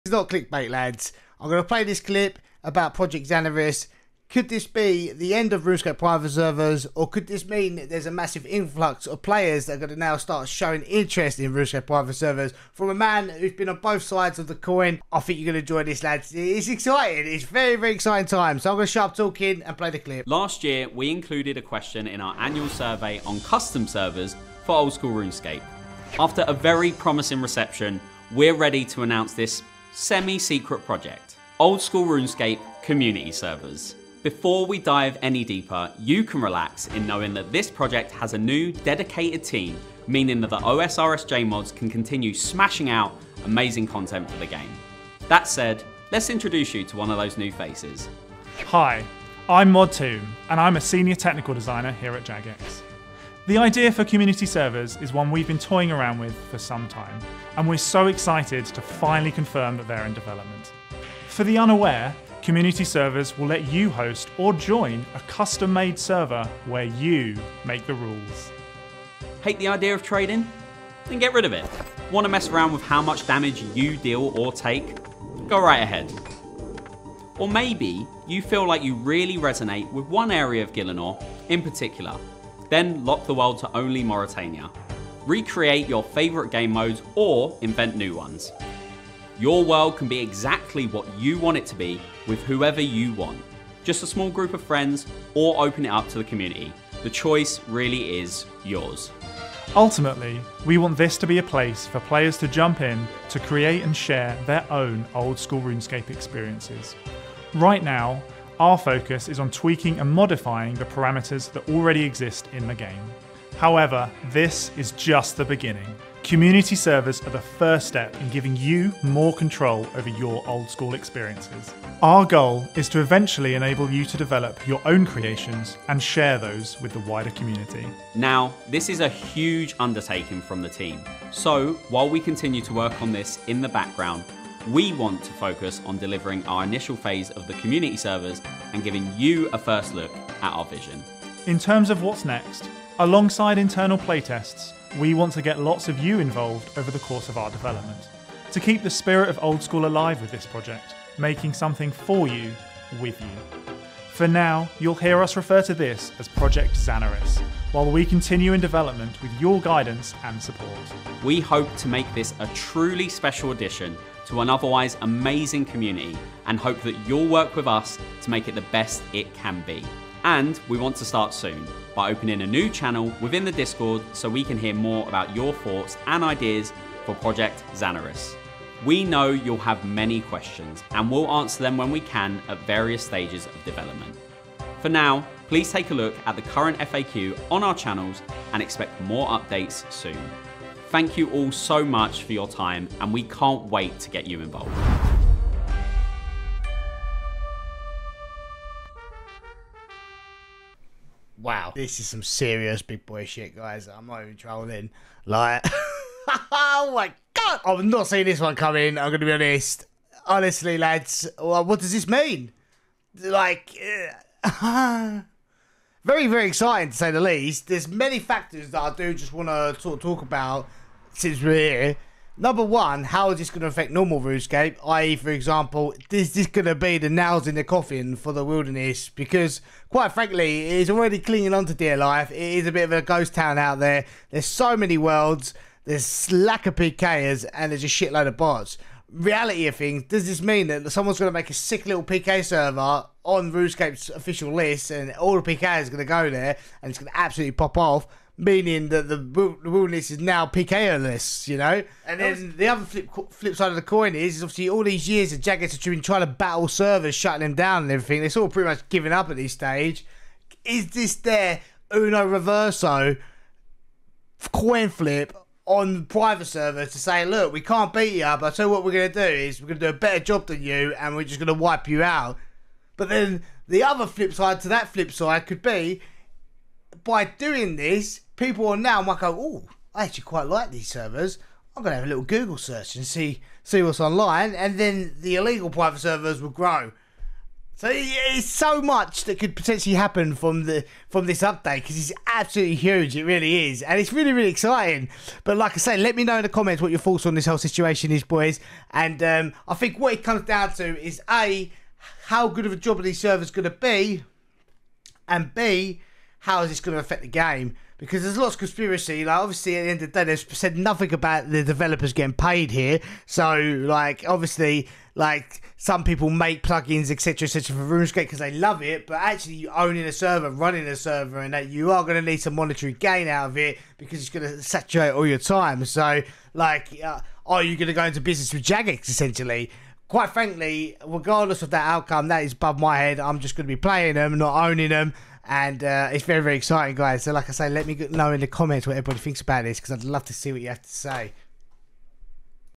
Could this be the end of Private Servers?
Definitely a surprising update to say the least! Very exciting for Runescape's future to say the least! What's your thoughts? Let you know in the comments!